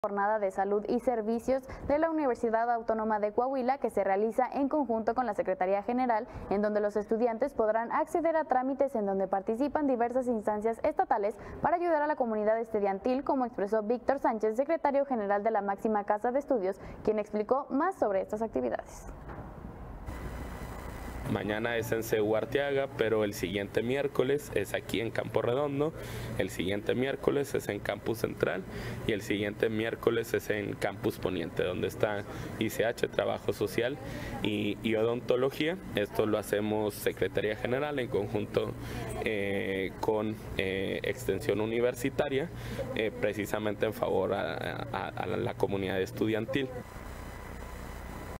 Realizarán jornada de salud y servicios
Coahuila | La UAdeC dio inicio a la Jornada de Salud y Servicio, programa con el que la comunidad estudiantil podrá acceder a diversos beneficios a favor de su bienestar.